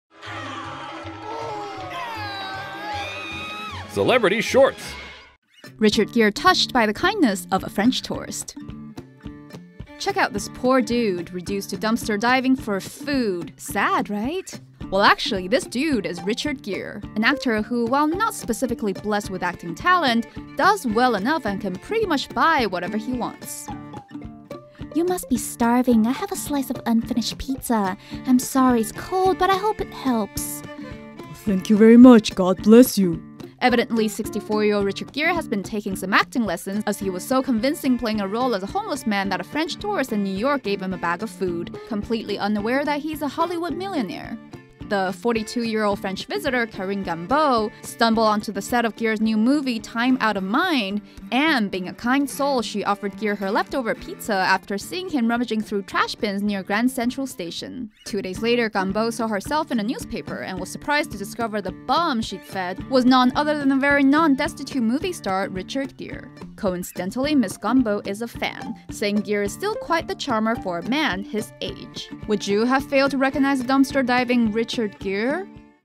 Richard Gere mistaken for homeless man, given leftover pizza by New York tourist
Richard Gere, 64, is a somewhat famous Hollywood star, but one French tourist failed to recognize the actor, mistaking him for a hobo and offering him her leftover pizza. The incident occurred as Gere was filming his new movie 'Time Out of Mind' in New York.

The visitor, Karine Gombeau, 42, did not know what she'd done until she saw herself in a newspaper. She said she had no idea the "bum" was actually Richard Gere.